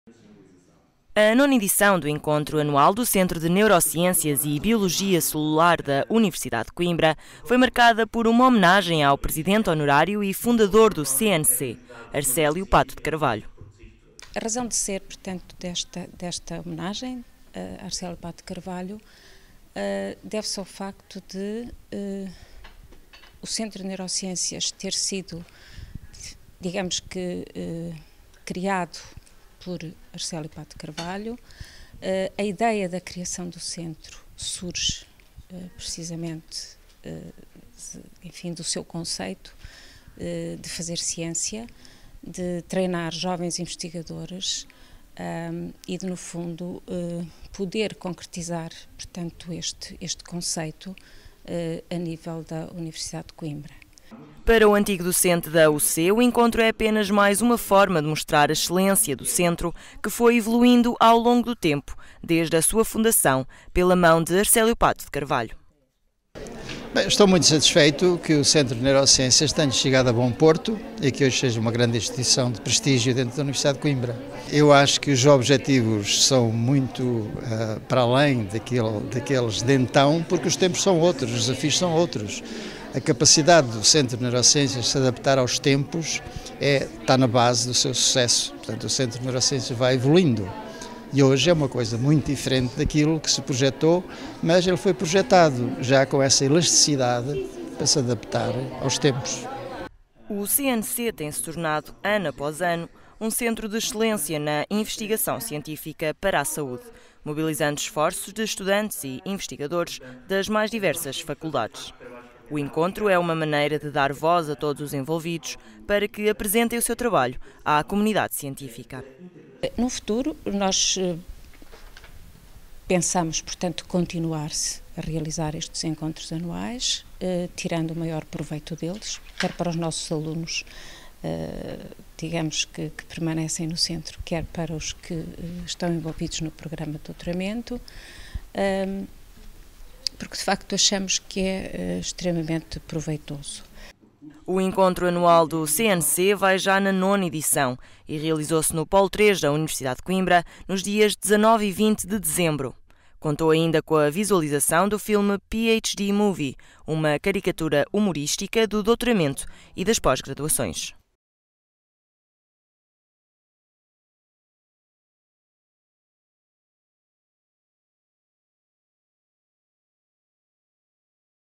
Arsélio Pato de Carvalho homenageado no CNC
O Centro de Neurociências e Biologia Celular da Universidade de Coimbra homenageou o seu presidente honorário e fundador, Arsélio Pato de Carvalho, no IX Encontro Anual do CNC. O evento é palco ainda para que estudantes e investigadores do centro apresentem os seus trabalhos à comunidade científica.